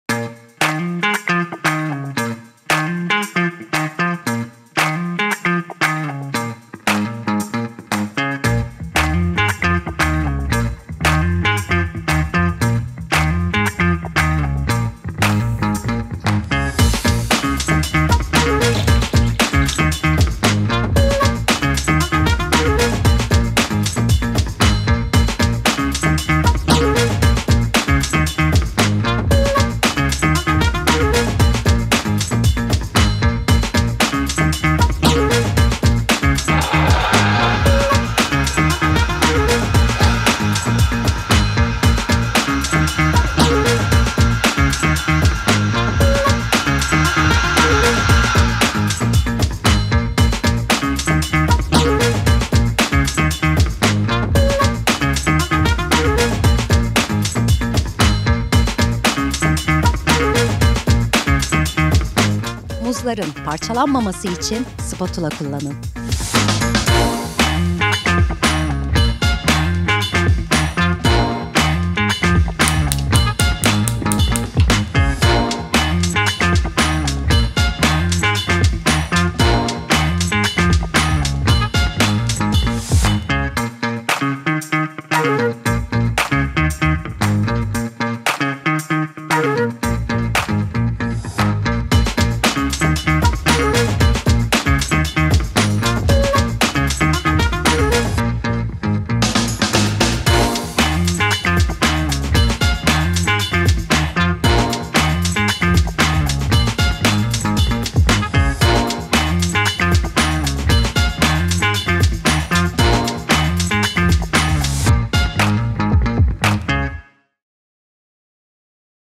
Muzlu Mini Krep Sandviç Nasıl Yapılır?
Farklılık arayanlar için, özel muzlu mini krep tarifini sizin için denedik.

Malzemeler:

2 adet muz
1 adet yumurta
2 yemek kaşığı şeker
1 su bardağı süt
200 g un
1 çay kaşığı kabartma tozu
3 yemek kaşığı sıvı yağ

Üzeri için:

Yaban mersini

Tarif:

1- Muzların kabuklarını soyup, halka şeklinde dilimleyin.
2- Arçelik Gurme Profesyonel Mutfak Makinesi’ nin içine yumurta, şeker, süt, un, kabartma tozu ve sıvı yağı ekleyip homojen hale gelene kadar çalıştırın.
3- Ardından muzları ekleyip spatula yardımı ile bir tur karıştırın.
4- Arçelik Nano Kaplamalı Ankastre Ocak’ ta yer alan tavaya karışımlı muz dilimlerini ekleyip arkalı önlü kızartın.
5- Muzlu mini krep sandviçlerinizi yaban mersini ile süsleyerek Arçelik Telve Türk Kahve Makinesi’ nde hazırlamış olduğunuz bol köpüklü kahve eşliğinde servis edebilirsiniz.

Not: Muzların parçalanmaması için spatula kullanın.

Afiyet Olsun!

Kullanılan ürünler;